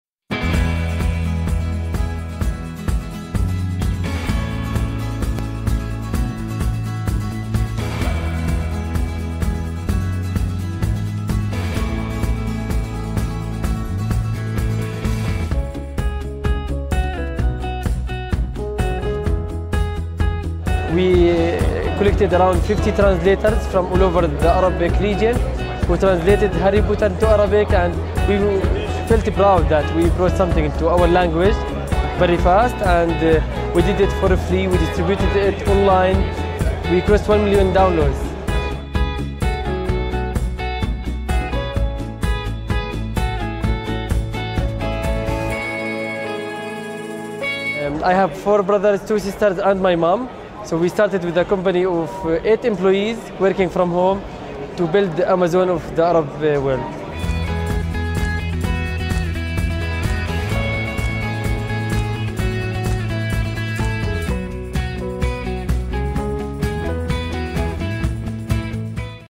أمازون للعالم العربي💪🏻😀🎊
أنشأ هذا الأردني أمازون العالم العربي. اكتشف كيف تمكن من فعل ذلك!💪🏻😀🎊